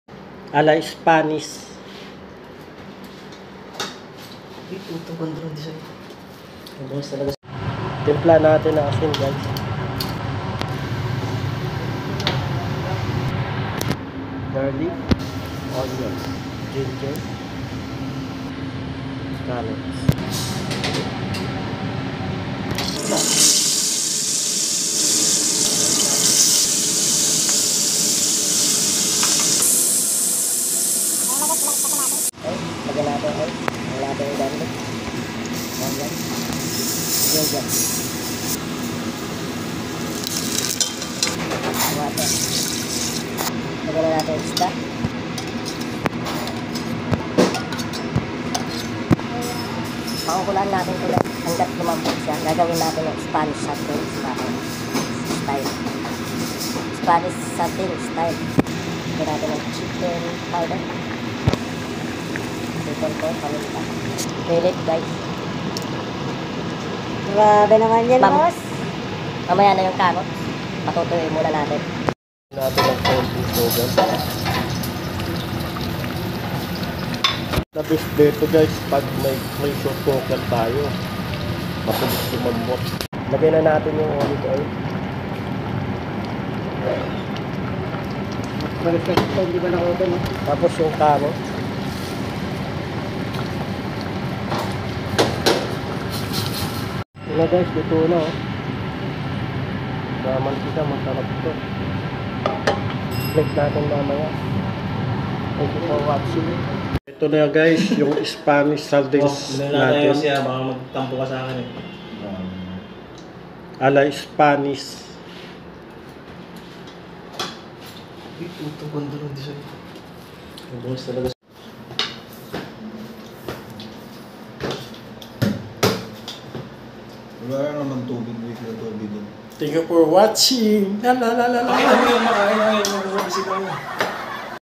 Paano Mag luto ng isada ala Spanish Sardines | tony chef
Paano magluto Ng isdang yellow pin ala  Spanish sardines...

tony chef